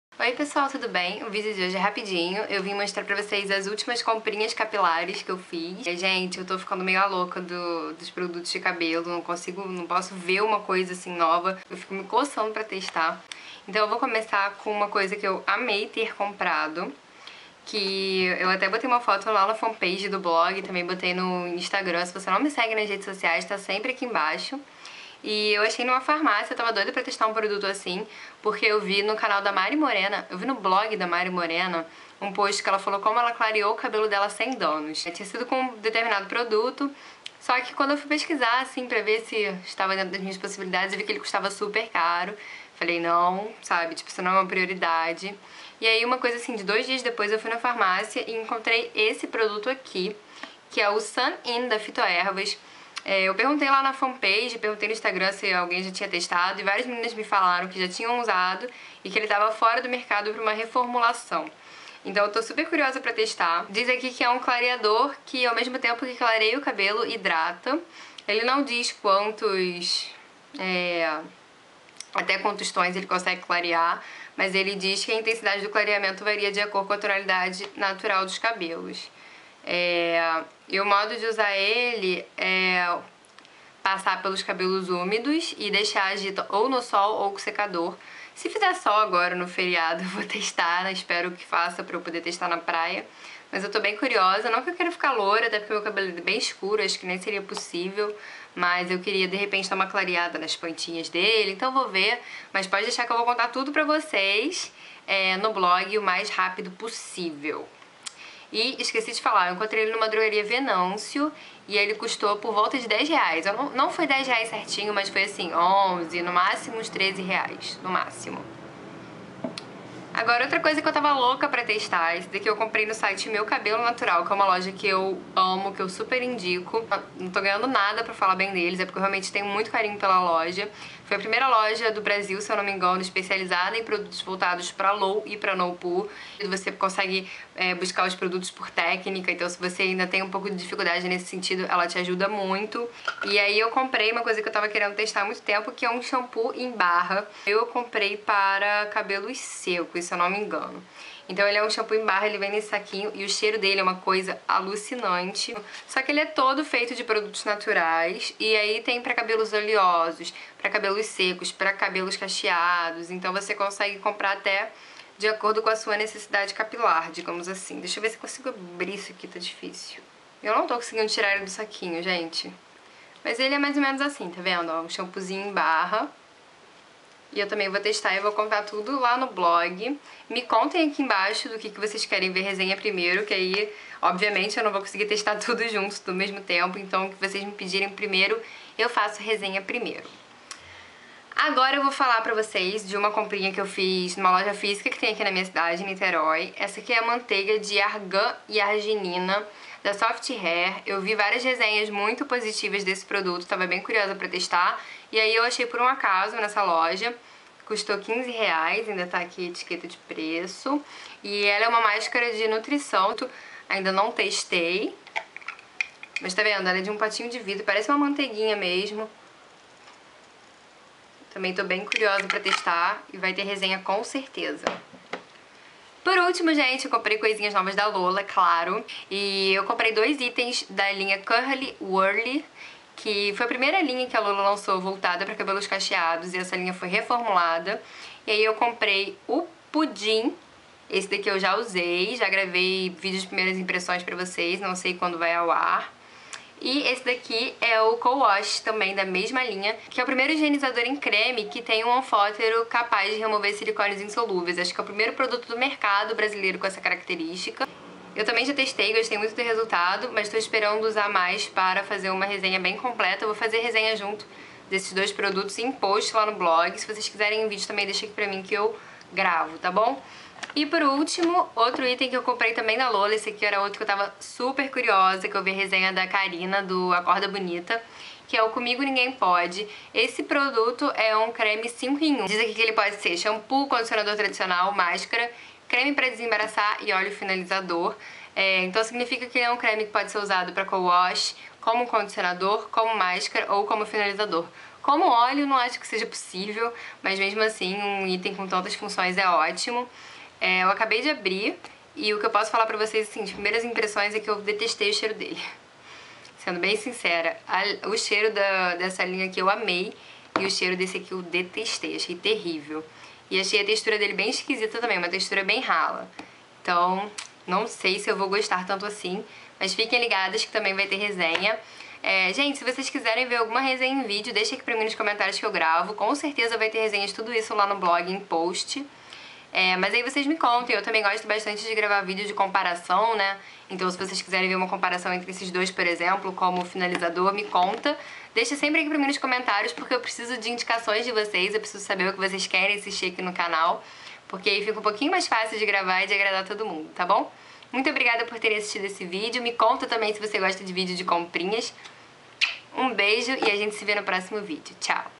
COMPRINHAS: PRODUTOS DE CABELO (LOWPOO)
Oi pessoal!
Vim mostrar pra vocês minhas últimas comprinhas de produtos para o cabelo.

Clique expandir para mais informações!

- Conheçam o blog! Posts novos todos os dias

Beijos e não esqueçam de se inscrever!